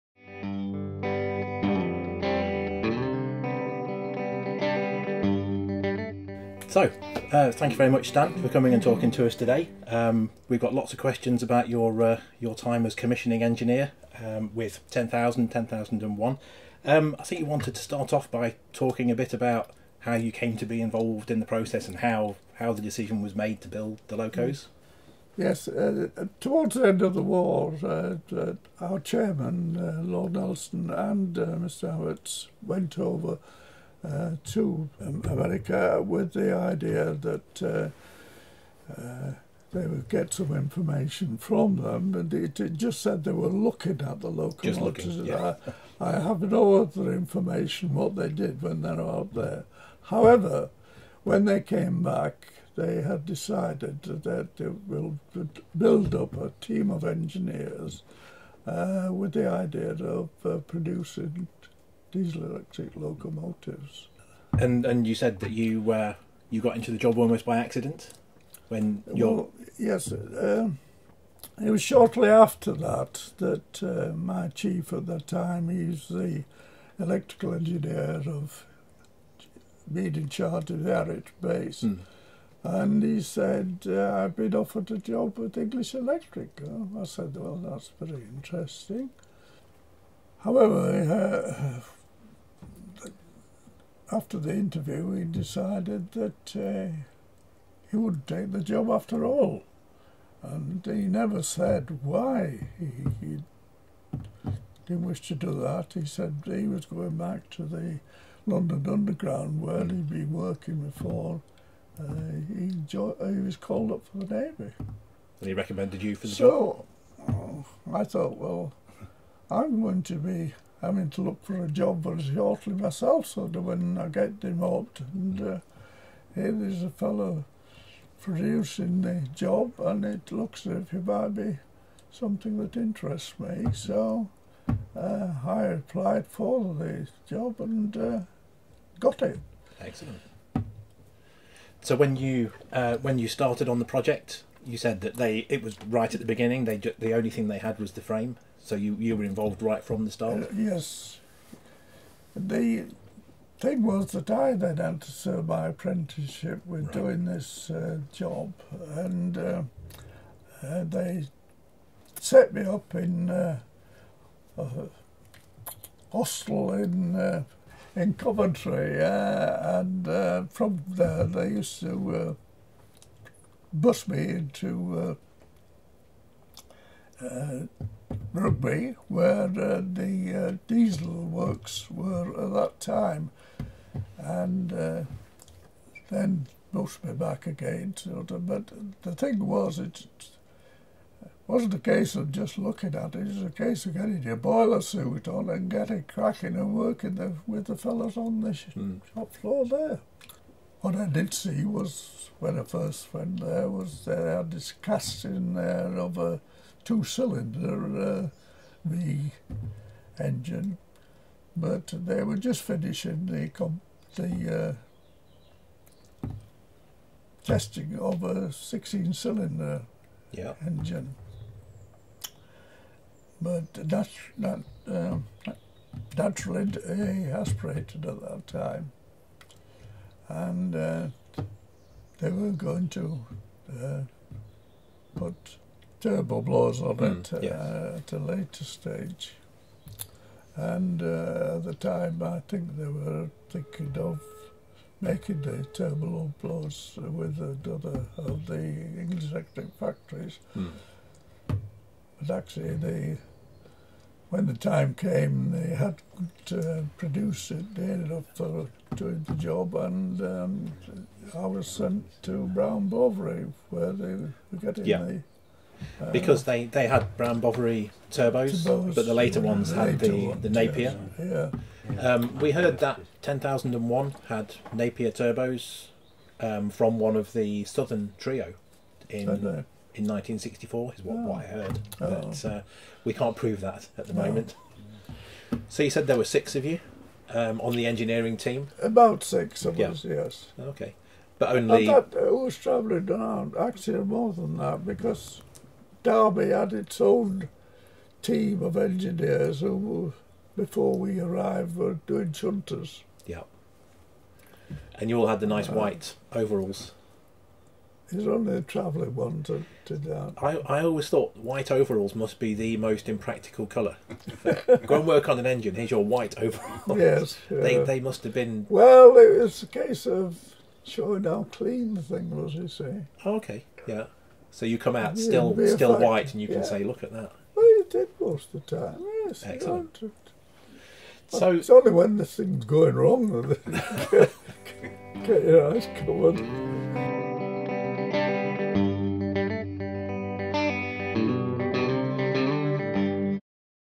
The Stan Fletcher Interview - Episode 1 - How I got the job with English Electric
Stan Fletcher was commissioning engineer for English Electric from 1947 and was involved with construction and testing of the LMS designed locos 10000 and 10001 until around 1950.

Stan, now aged 98, is President of the Ivatt Diesel Re-creation Society, who aims to reconstruct 10000 using an original English Electric 16SVT power unit built in 1947.

See our website http:LMS10000.co.uk

This episode is 1 of 5 to be released as monthly episodes from 01/02/2019